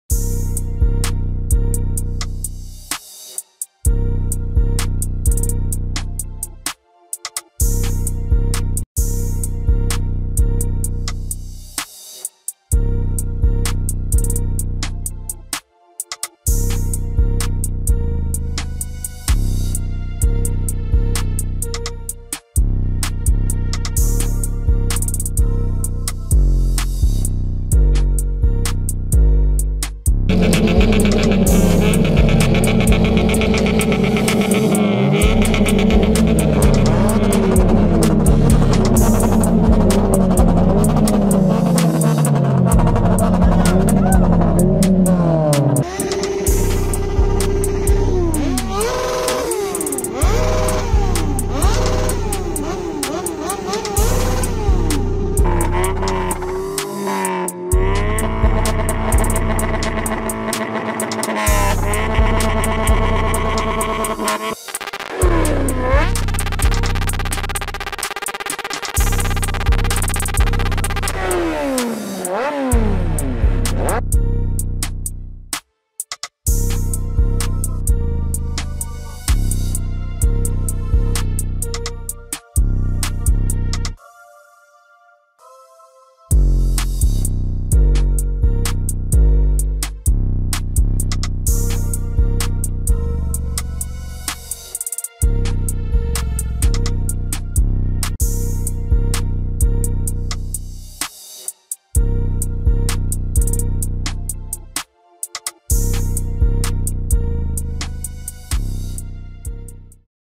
Revving Up Excitement: Highlights from the DIKhan 2nd Annual Auto Show
The 2nd Dera Ismail Khan's Grand Auto Show was a thrilling event that showcased some of the most innovative and exciting vehicles in the automotive industry. From classic cars to futuristic modified vehicles, attendees were treated to a wide range of displays and activities that celebrated the passion and creativity of Car, Jeep & Bike enthusiasts.